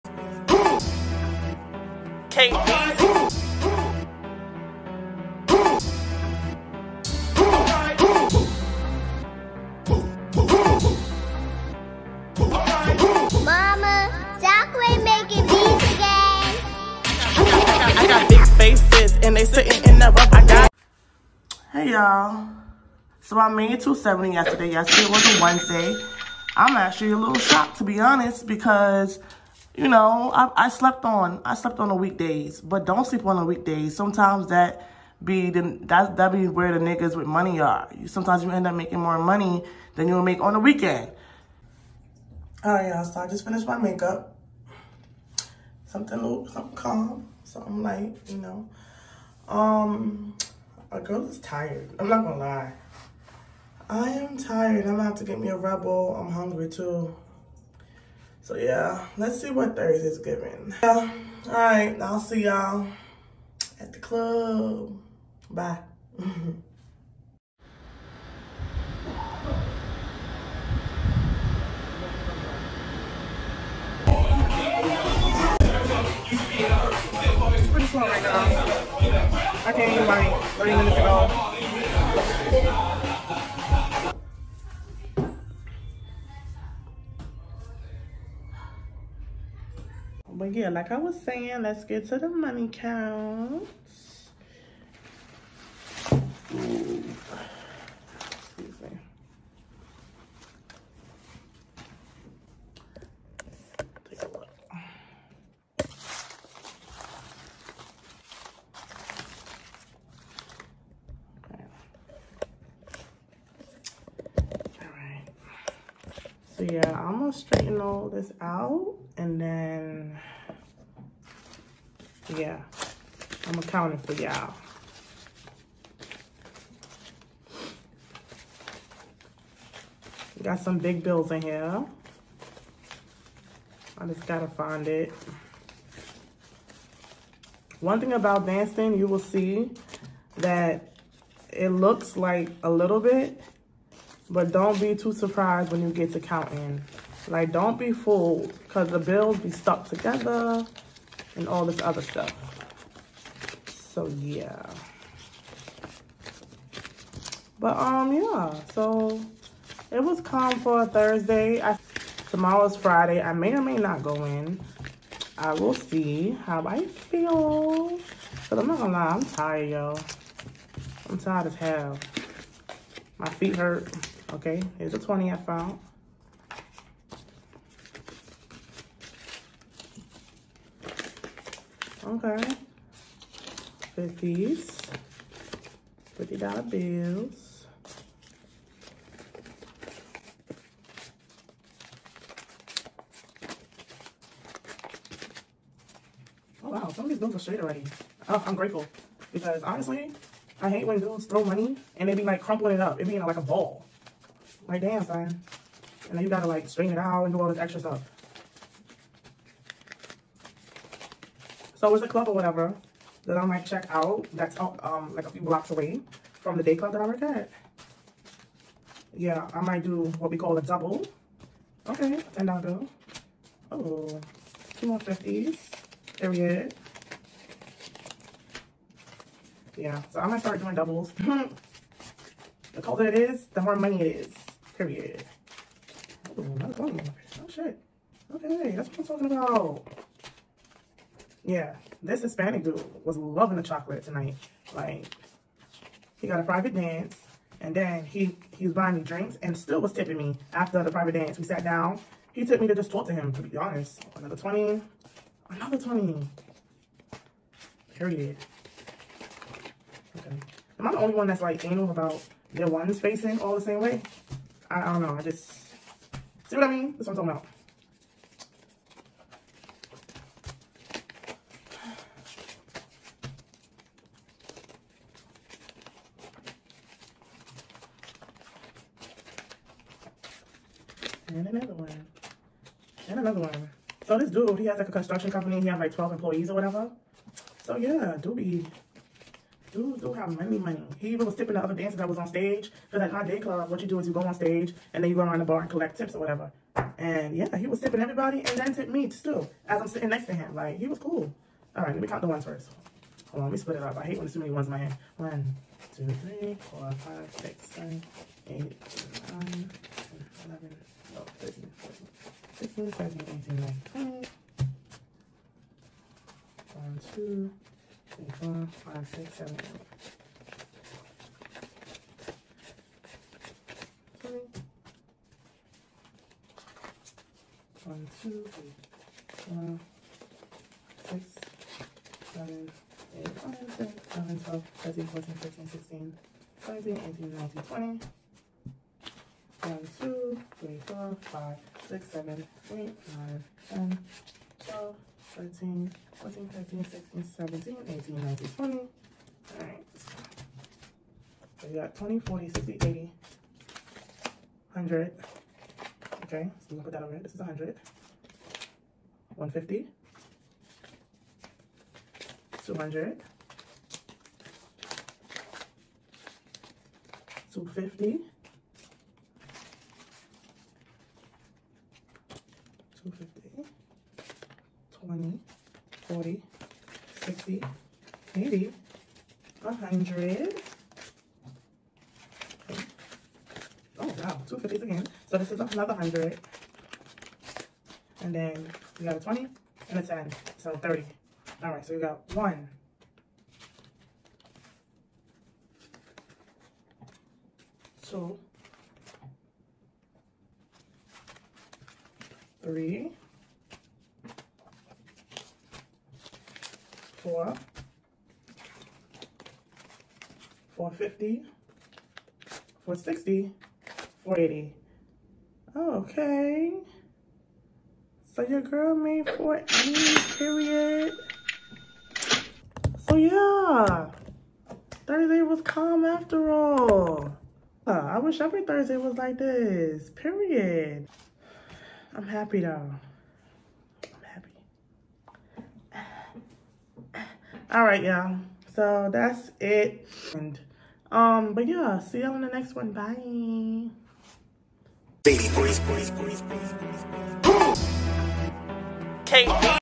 NYC STRIPPER VLOG | Day In The Life/Come To Work With Me | Money Count | Tall Dancer Edition
SOCIALS

TRILLER: @KutieBlue